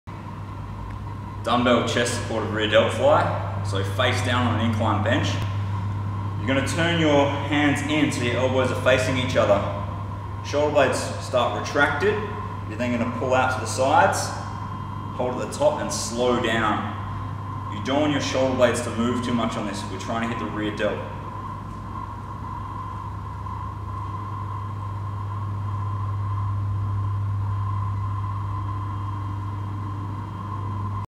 DB Chest Supported Rear Delt Fly
Step 1: Set up an adjustable bench to a 45° angle and lie face down with the mid part of the chest overhanging the top of the bench.
Step 2: Turn the dumbbells in so that the elbows are facing each other.
Step 3: Pull the dumbbells out to the side squeezing through the rear Deltoids with a small hold at the top of the exercise.
Step 4: Under Control, lower the weight back down to start position.
Step 5: Repeat the full process.